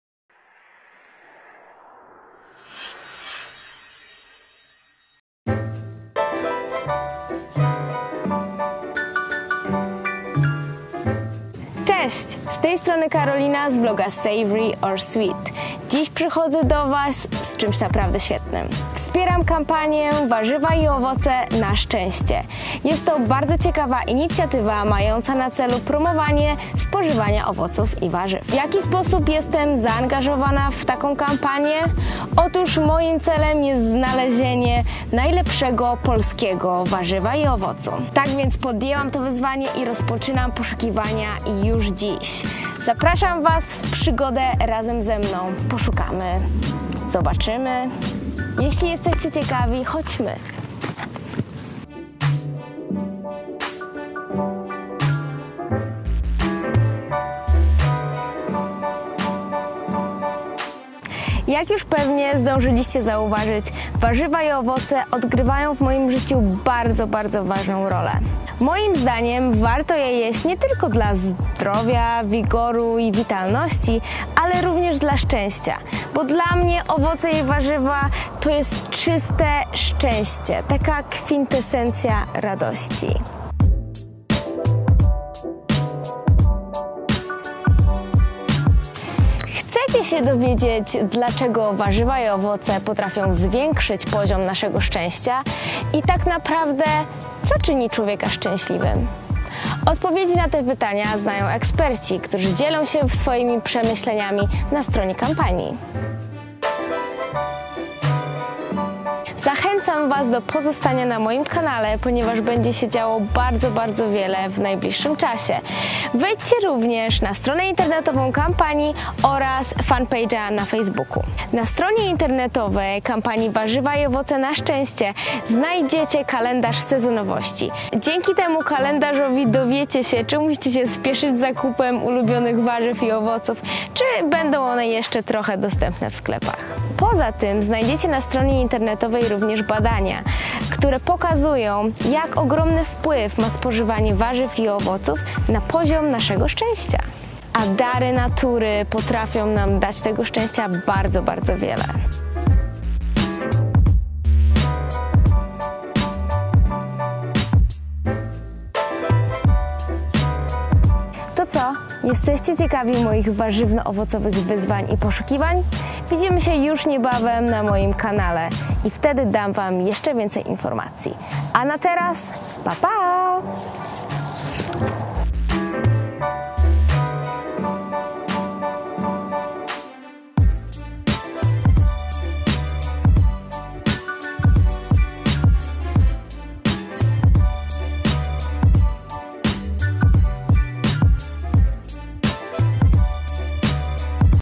WARZYWNO-OWOCOWE WYZWANIA - zapowiedź | Savory or Sweet?
KUP MOJĄ KSIĄŻKĘ "Wegańskie Słodycze - kultowe, zdrowe, bezglutenowe"

Moi mili! Wspieram kampanię "Warzywa i Owoce -  na szczęście" i w związku z tym na moim kanale pojawi się cykl wyzwań WARZYWNO-OWOCOWYCH!

O co w tym wszystkim chodzi? Moim zadaniem jest znalezienie najlepszego POLSKIEGO WARZYWA I OWOCU!

Jesteście gotowi by wyruszyć ze mną w podróż po świecie darów natury? Więcej informacji już niebawem. Tymczasem polecam poczytać więcej o kampanii, ponieważ jest to świetna inicjatywa: